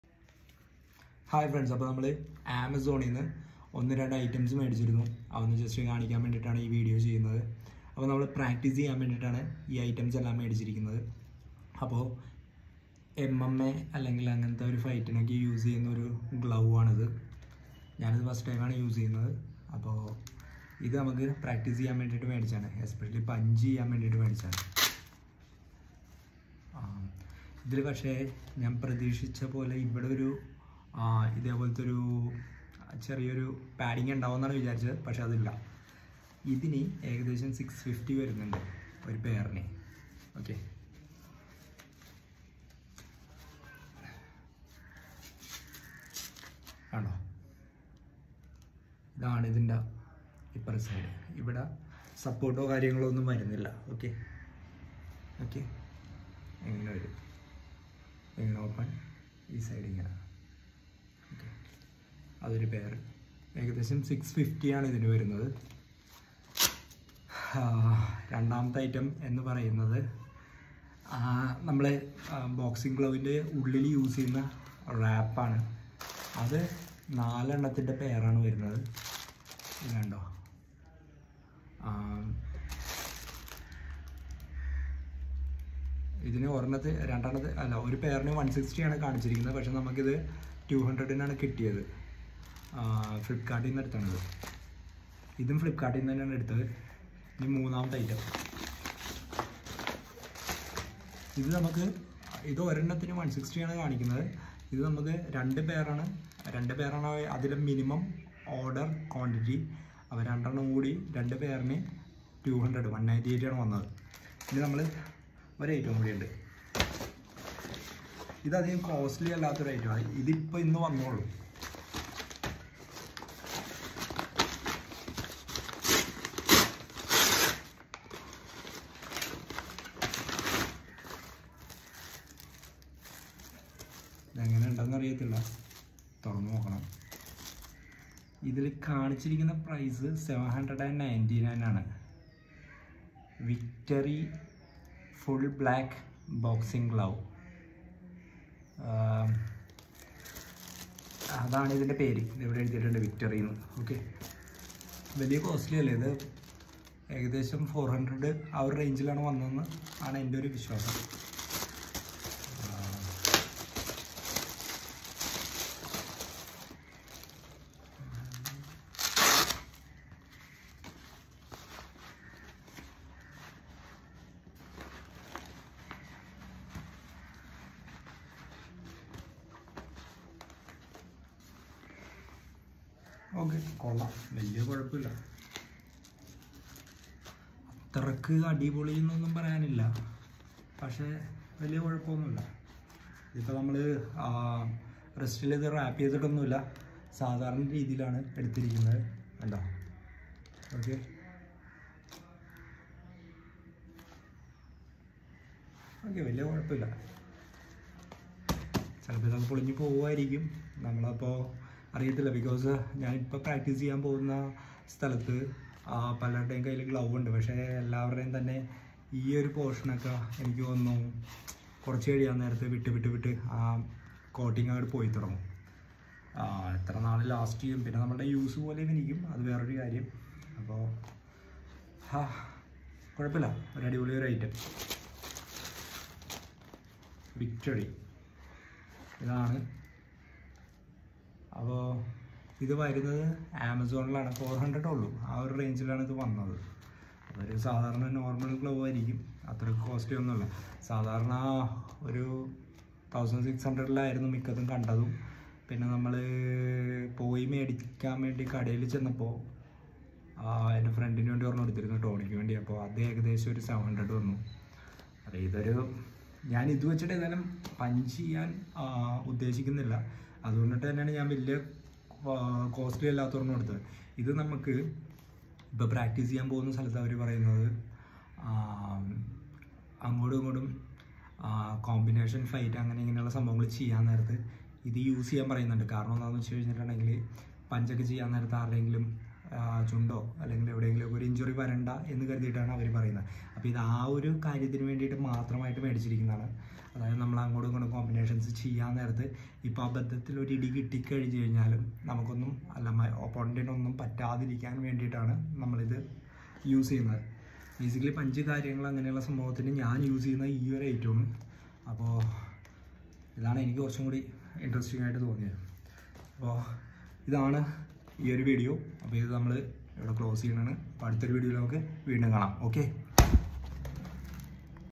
Boxing Gloves / Boxing wraps / MMA gloves
These are some cheap boxing accessories from Amazon.  I am using it regularly for the past 2 months and no issues at all even with very rough use.
Boxing Glove Rs 399.

MMA Gloves Rs 664.

Wrap Rs 198.